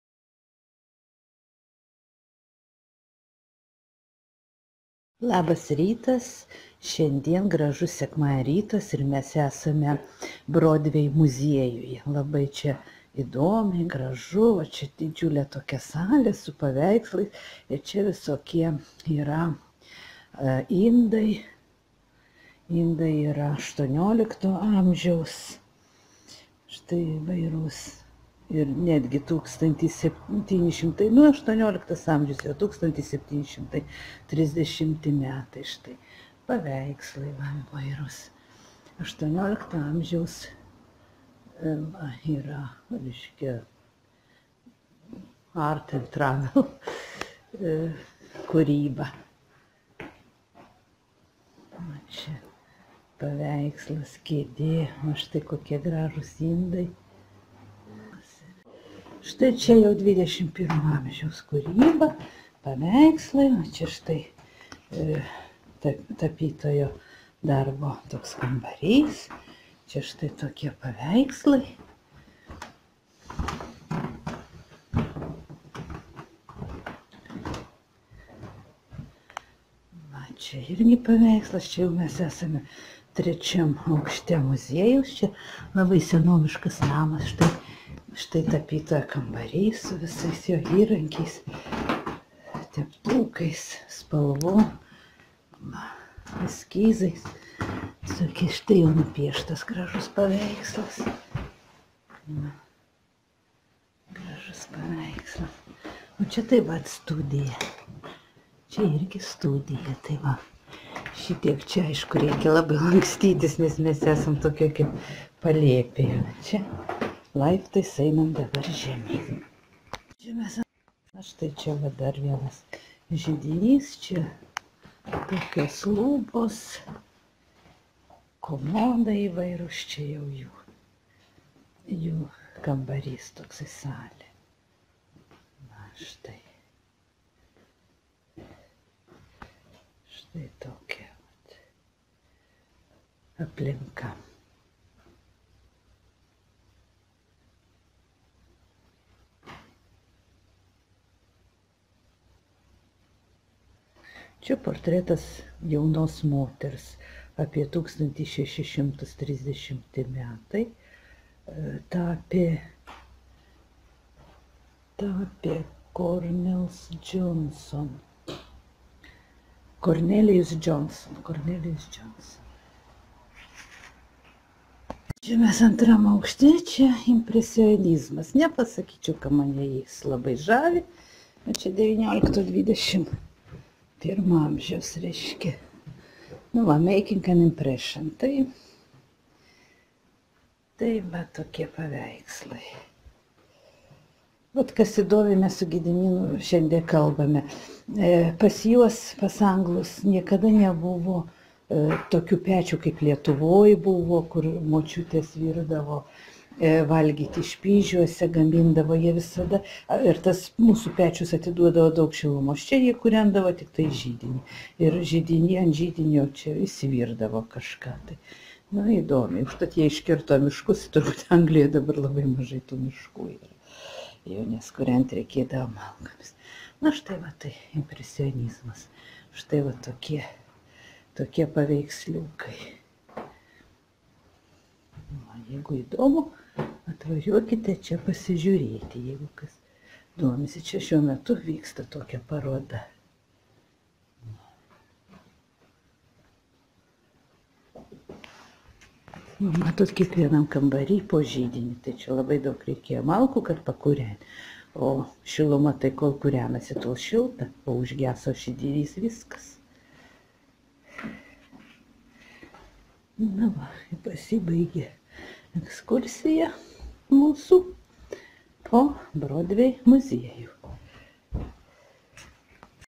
Broadway muziejus
Gražiai praleistas savaitgalis. Aplankytas Broadway muziejus.